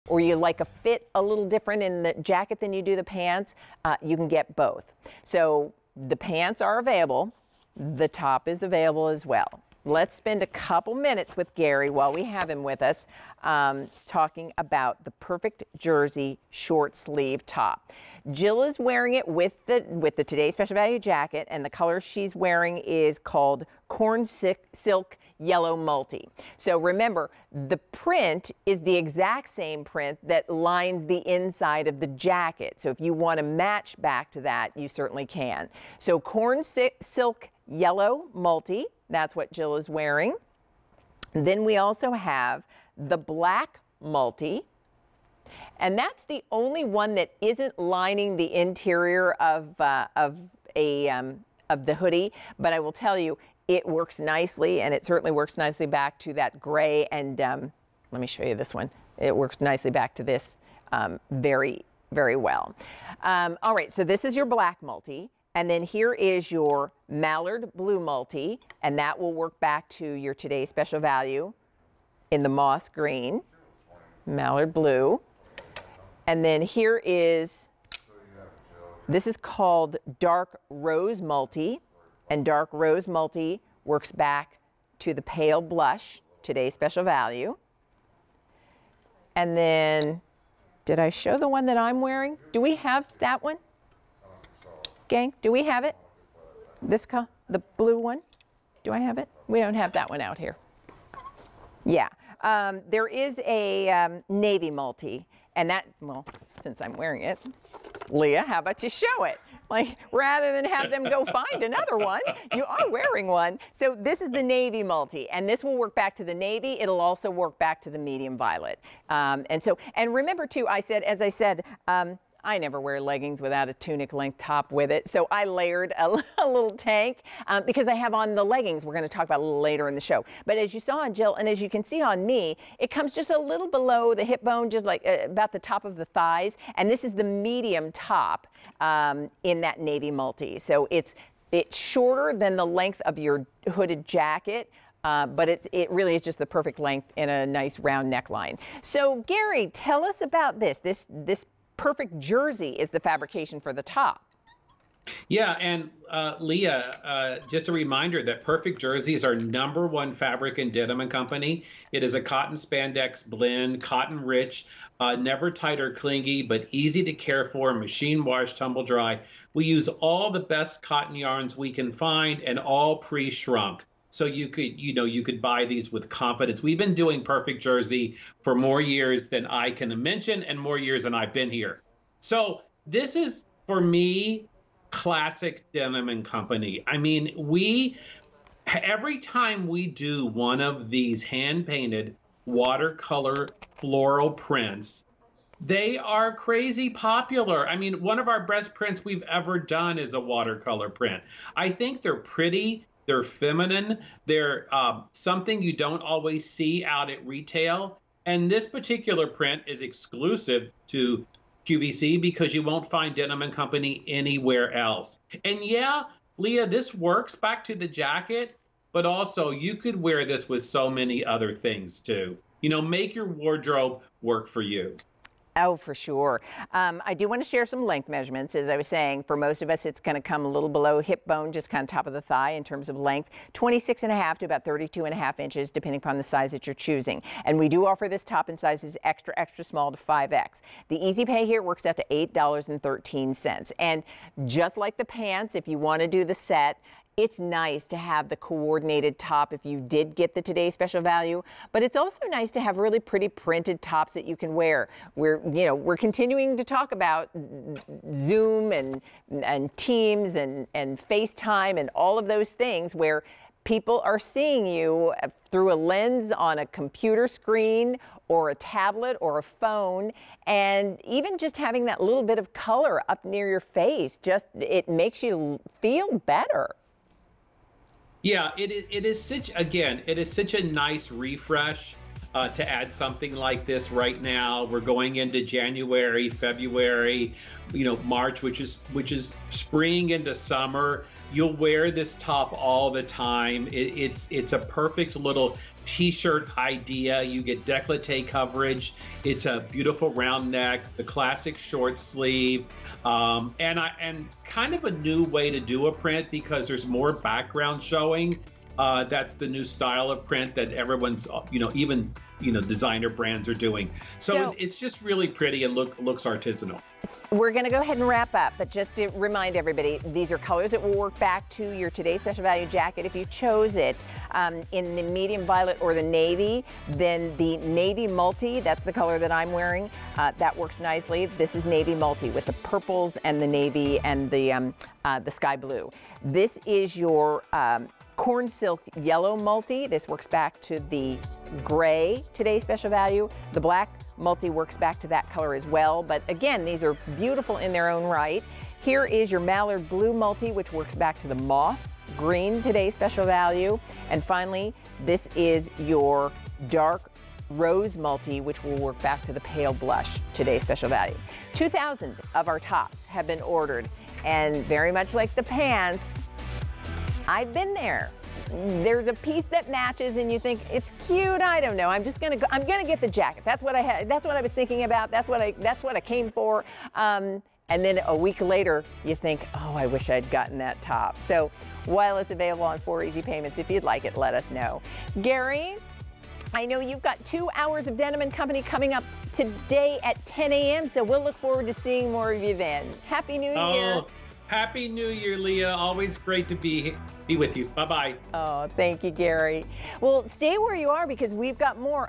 Denim & Co. Printed Perfect Jersey Short-Sleeve Top on QVC
Anything but basic, this Perfect Jersey top checks all the boxes -- cute, comfy, and designed with a playful print that wakes up your wardrobe. From Denim & Co.&reg; Fashions.